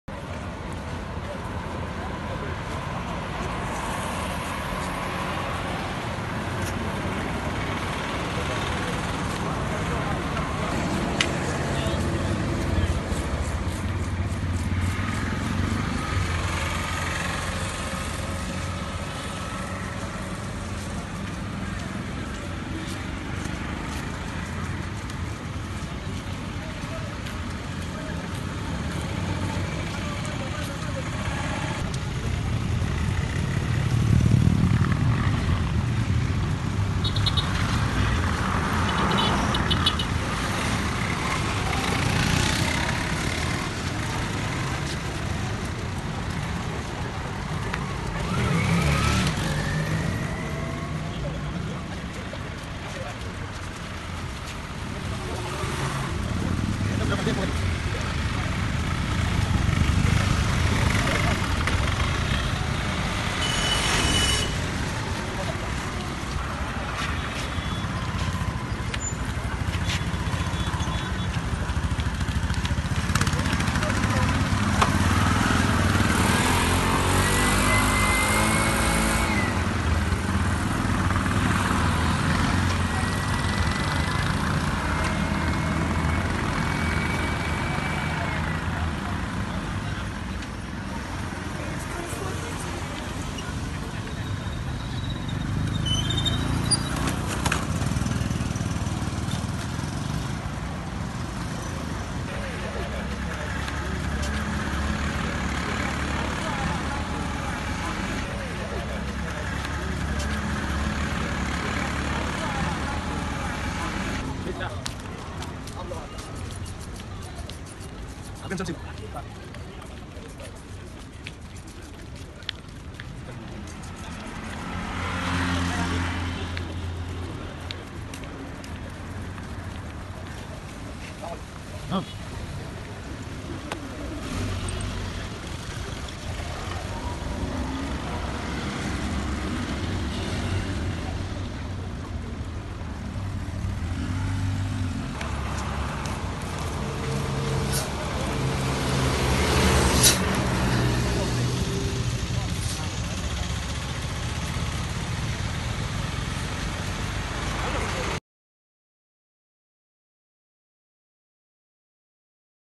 Commuters rush to board ferry at Likoni before curfew starts
Commuters in Mombasa rush to board the ferry before the 7pm curfew deadline